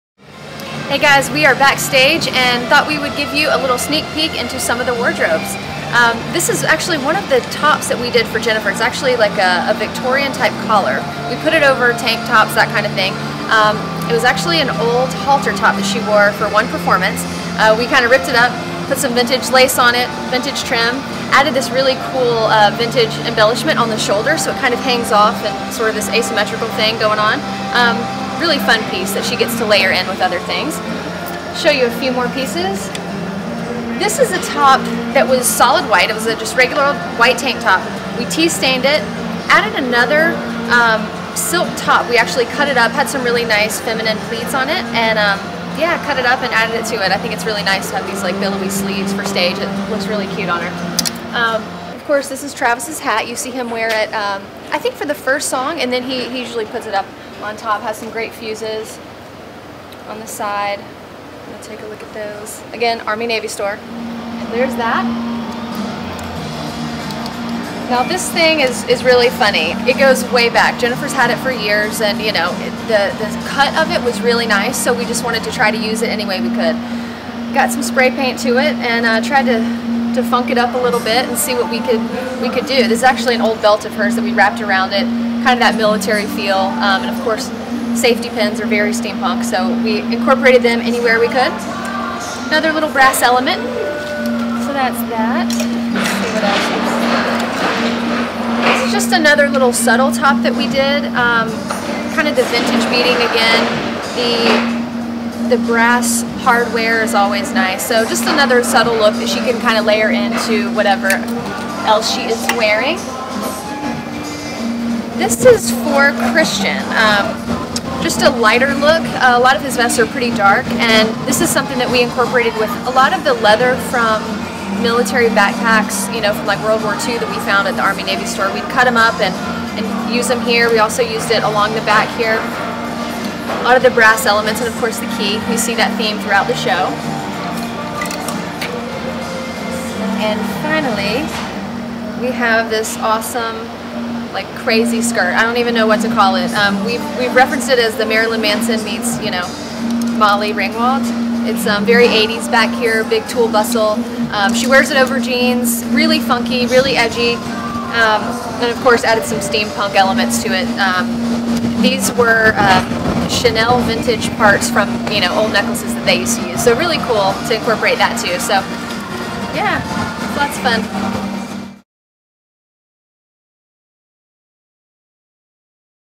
Sugarland TV: The Incredible Machine's costume designer talks steampunk fashion (Part 2)
The Incredible Machine tour costume designer Tiffany Terranova opens Jennifer and Kristian's wardrobe cases to give you an up-close look at the pieces they're wearing on stage.